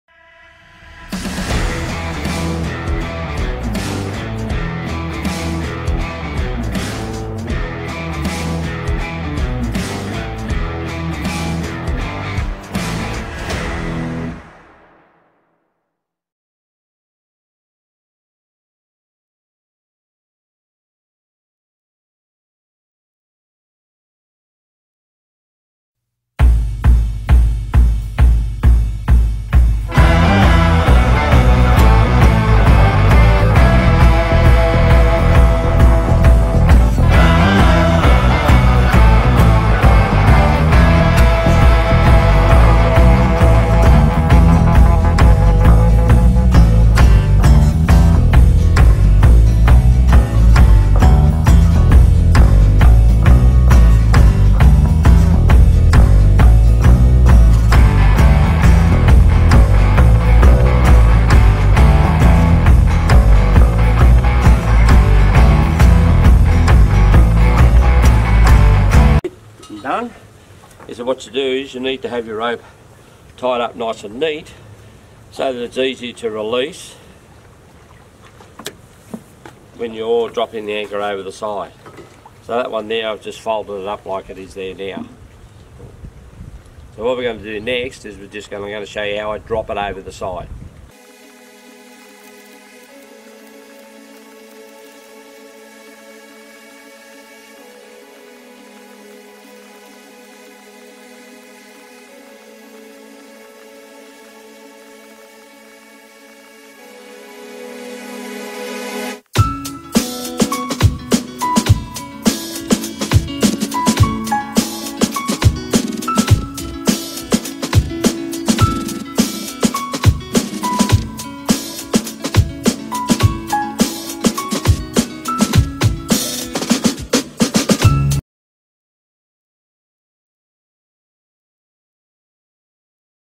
# Anchoring Techniques Part 2 - How not to store your anchor rope
Make sure that when you store your anchor rope that its not a mess like I have done here.